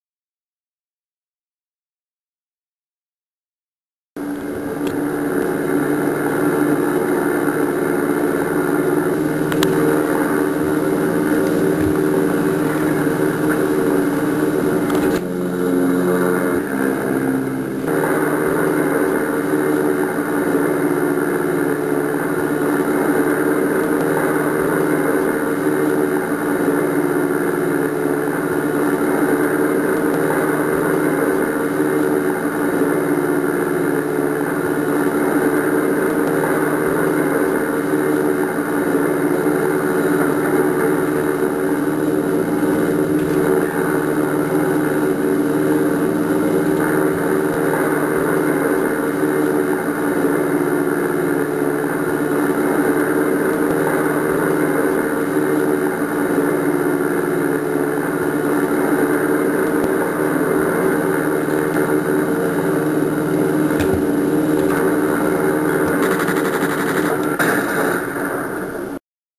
Stationary Plane Flight Test
Trying to make stationary plane look like it is in motion. My lego Plane lol.